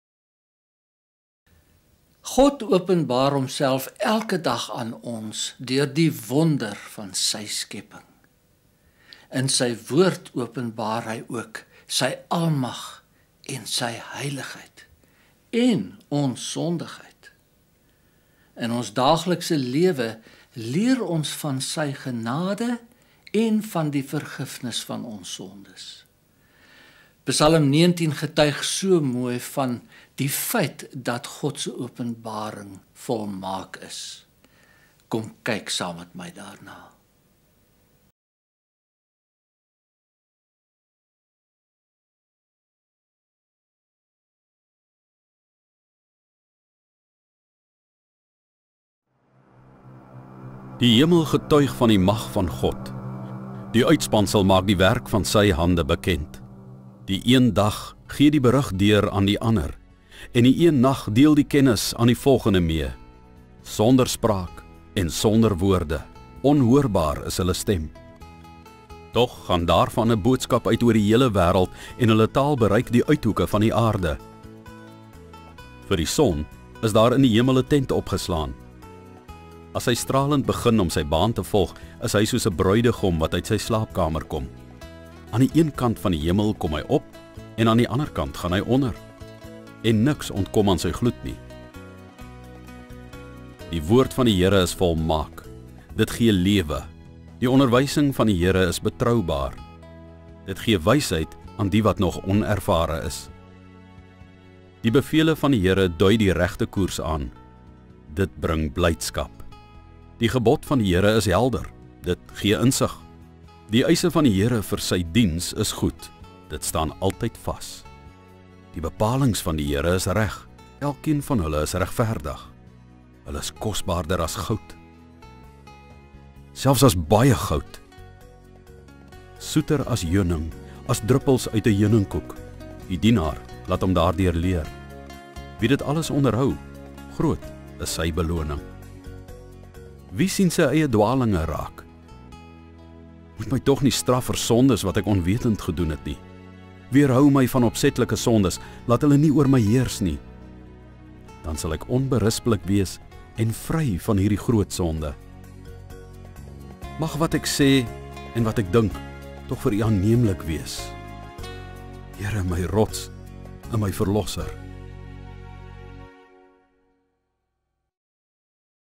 Die Woord Lewe 2 - Psalm 19.mp4
Hierdie is die tweede in 'n reeks van 9 videos onder die titel: Die Woord Lewe.
Die aanbieder is Jean Kok en die voorleser is Johan Rademan. Die Bybelteks kom uit die  1983 Afrikaanse Vertaling en dit word met videobeelde, wat in Suid-Afrika en in Namibië geskiet is, ondersteun.
Ons het vier jaar aan hierdie projek gewerk en dit is gedurende die jaar 2004 voltooi.